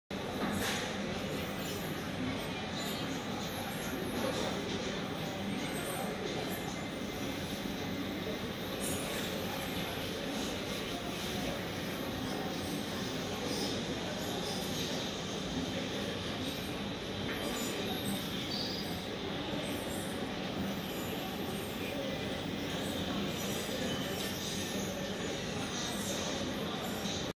Kuning Beling², Luster Nembak 🌟, Buat Sultan 😅 (Natural Yellow Sapphire)
Natural Yellow Sapphire
Warna: Yellow
Berat: 6,62 crt
Dimensi: 9,98 × 9,96 × 6,65mm
H

Toko: MUEZZA GEMS & JEWELRY
Alamat: Blok BKS 096, Lt. 1, Jakarta Gems Center (JGC), Rawabening, Jatinegara, Jakarta Timur, DKI Jakarta